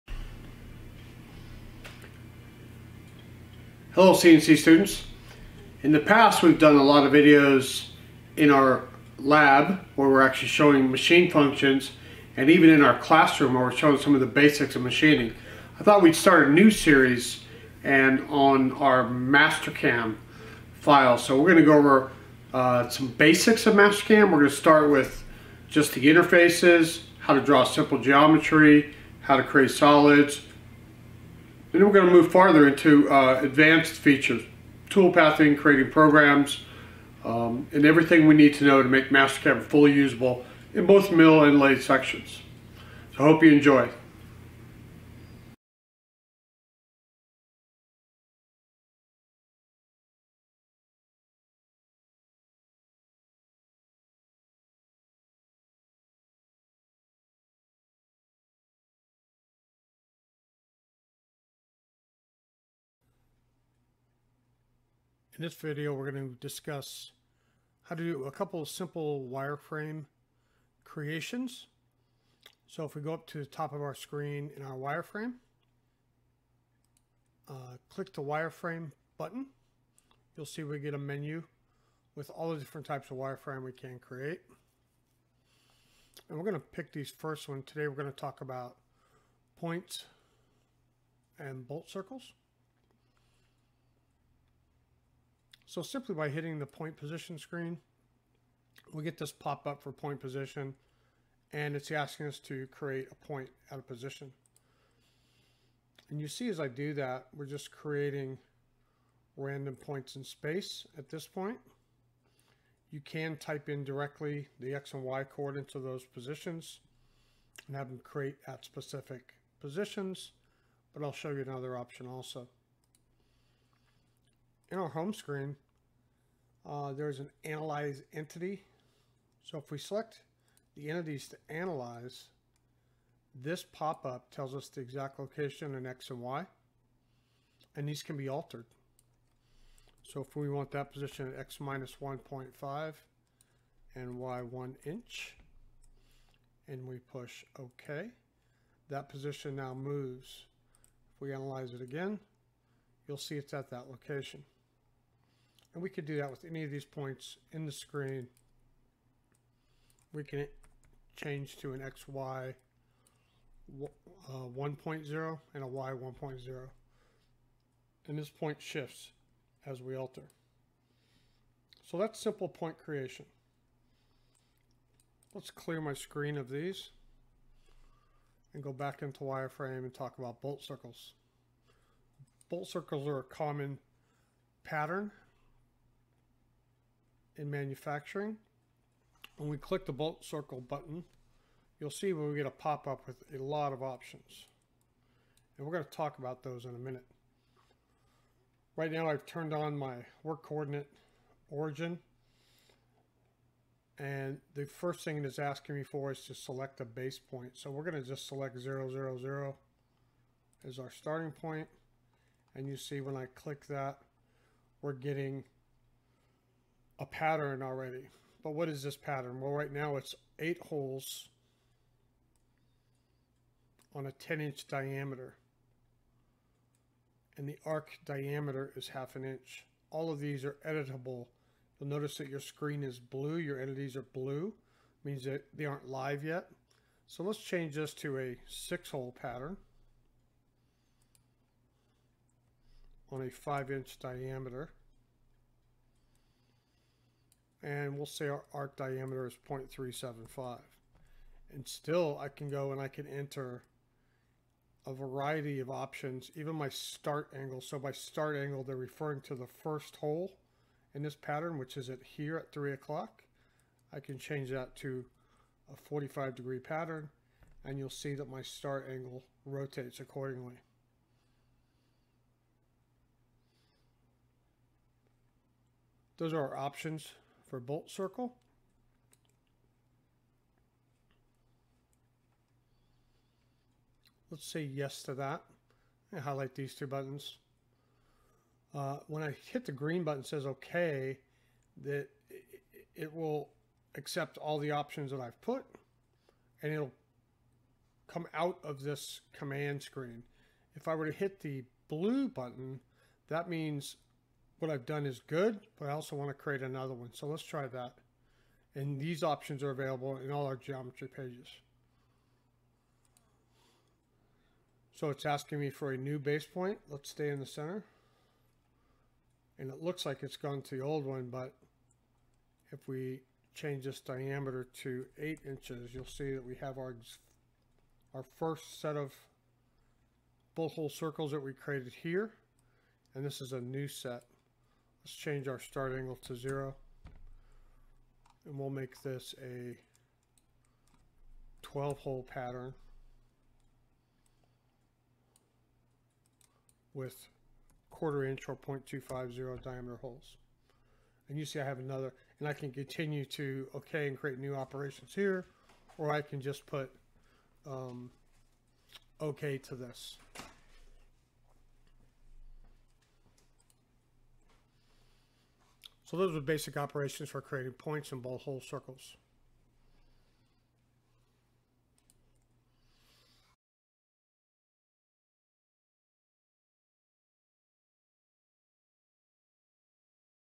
Mastercam Points and Bolt Hole Circles
A quick look into creating and editing points and bolt hole circles